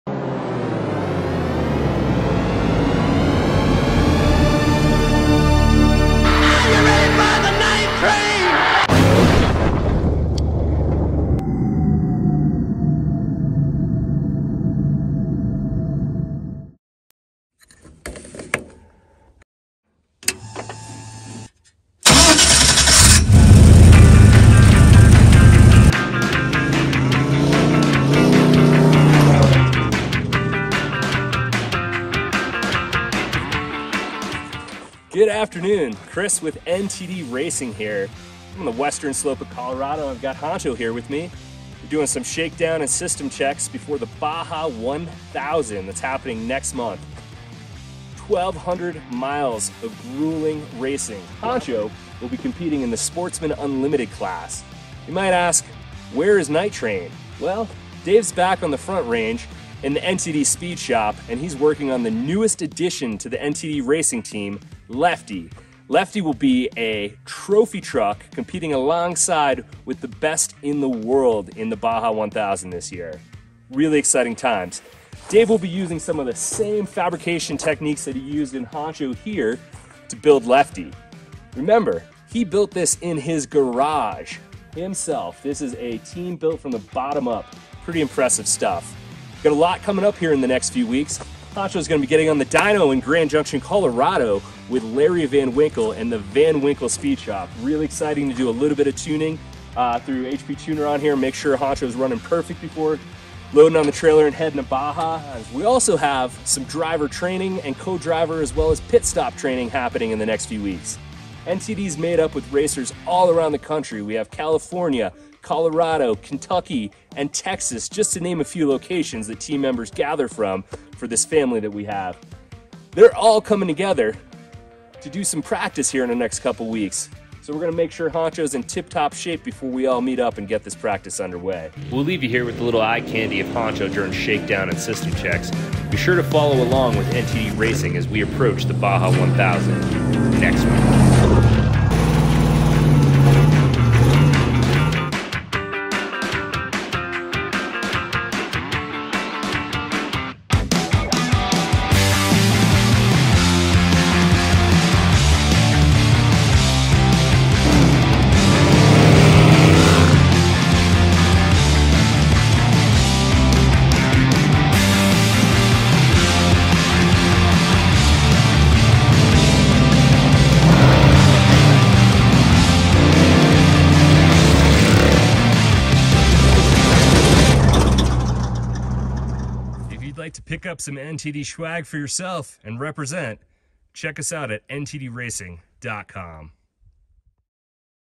Final prep days for the Baja 1000
Kris from NTD Racing takes HONCHO! to Colorado for some final Baja 1000 prep.  This will be the 8th race for this Jeep J10 and our 4th attempt at finishing the worlds longest continuous off road race...The Baja 1000.